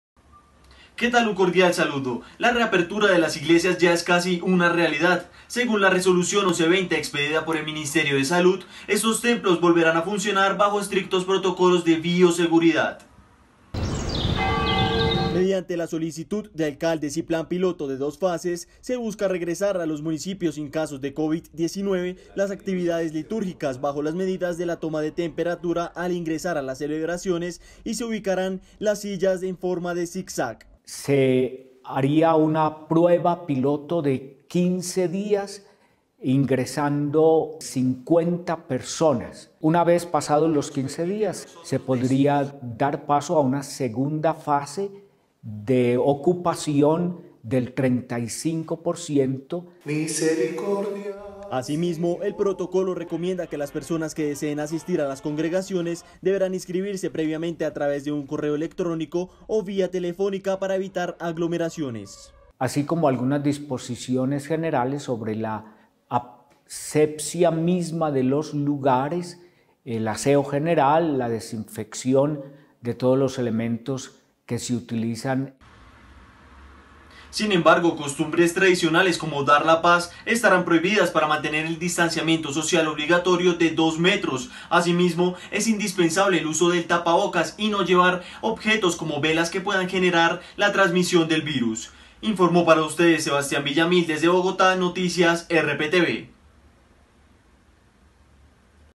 Así es el plan piloto para la reapertura de iglesias | TV5 Noticias
Nación | La reapertura de las iglesias ya es casi una realidad, según la resolución 1120 expedida por el Ministerio de Salud estos templos volverán a funcionar bajo protocolos de bioseguridad. Mediante la solicitud de alcaldes y un plan piloto de dos fases se busca regresar a los municipios sin casos de covid-19 la actividades litúrgicas bajos las medidas de la toma de temperatura.